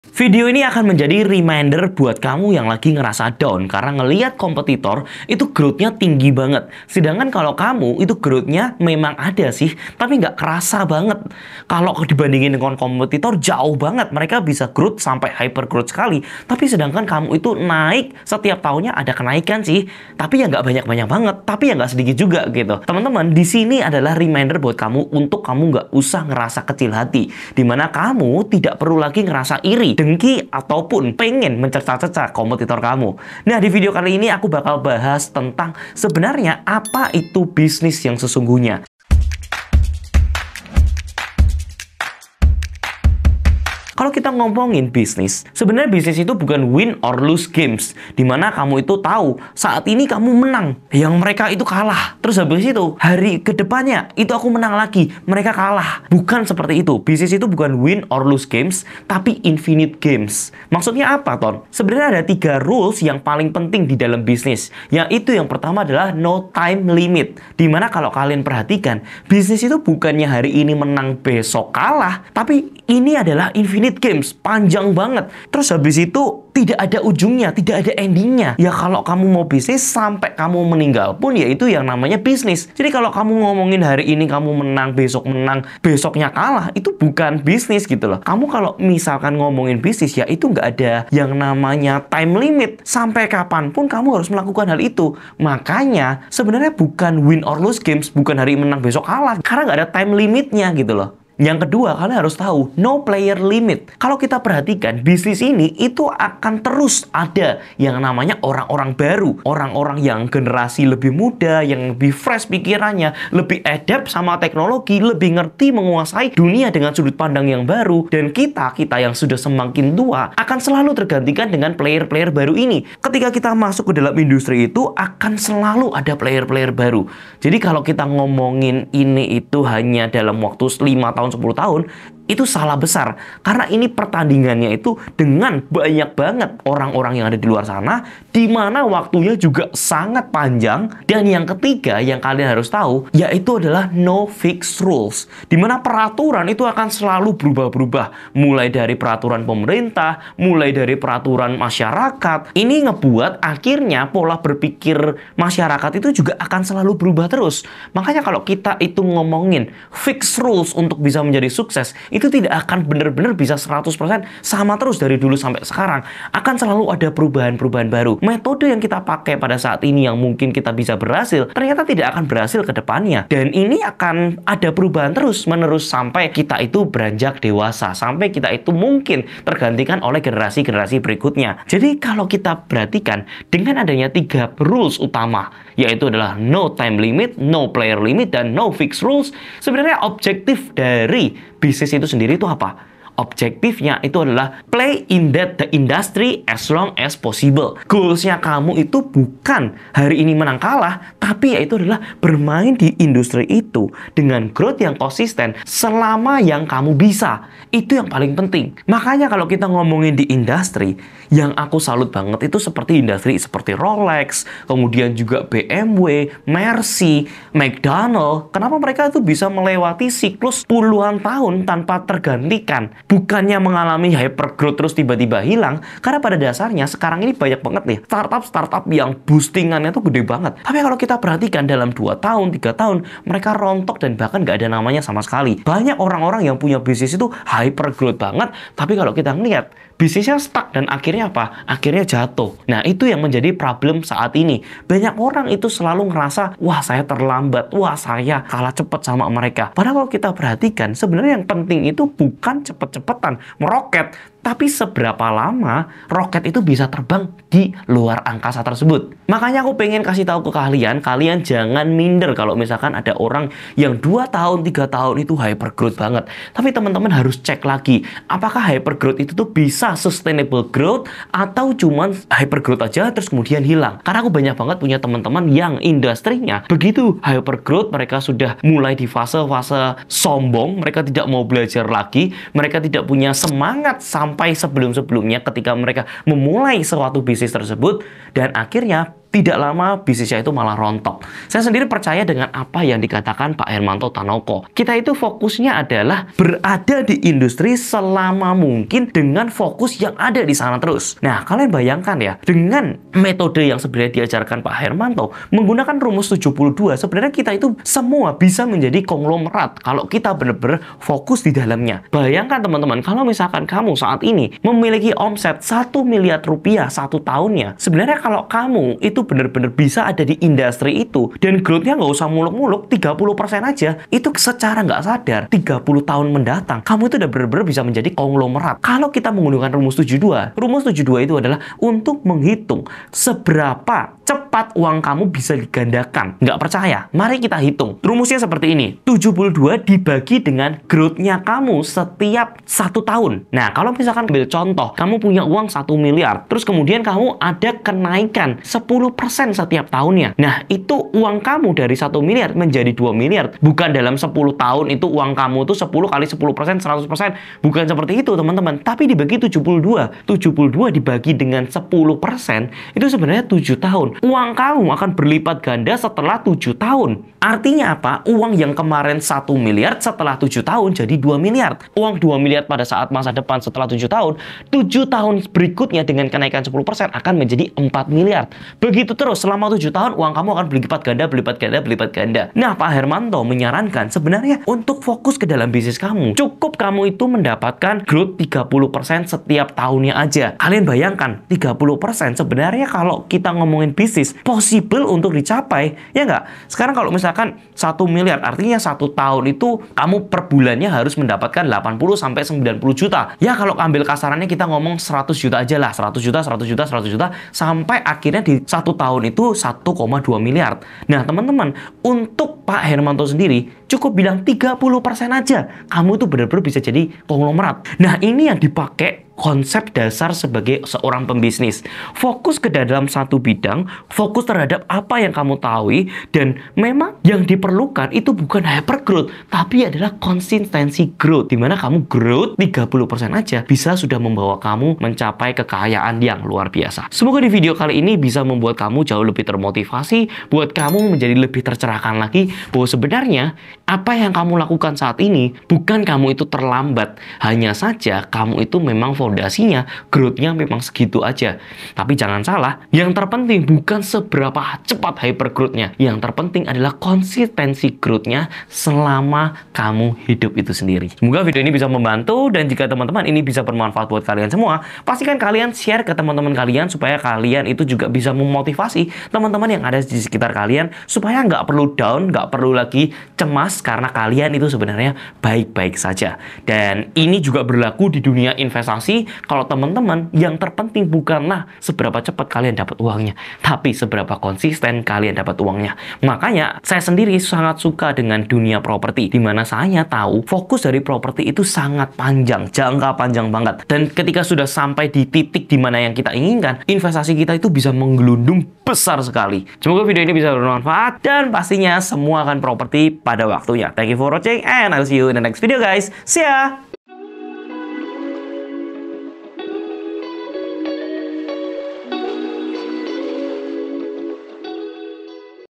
3 Hal WAJIB Tau sebelum Bisnis/Investasi buat Millenial
Thankyou udah nonton video ini sampai selesai..
dan insight baru apa nih yang kalian dapet dari pembahasan kali ini? yuk obrolin di kolom komentar bawah ya..
Chapters
00:00 Introduction
00:44 Bisnis = Infinite Games
01:04 Rule 1: No Time Limit
01:41 Rule 2: No Player Limit
02:21 Rule 3: No Fix Rules
03:13 Tujuan Bisnis = Play in the industry as long as possible
05:28 Rumus 72
06:58 Konsep Dasar Pebisnis
Dan buat temen temen yang pengen belajar lebih detail tentang property
kalian bisa juga join di Workshop saya selanjutnya,
untuk mengetahui ketersedian seat, tanggal dan Harga Workshopnya,
kalian bisa akses ke sini ya :